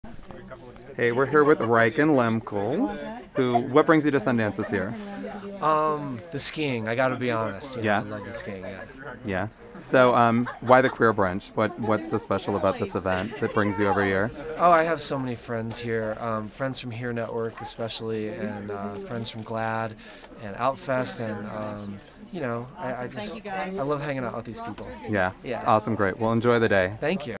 GayWired Live from Sundance - Reichen Lehmkuhl
Dante's Cove and Amazing Race star Reichen Lehmkuhl took a break from the slopes to stop by the Queer Brunch and support his filmmaker friends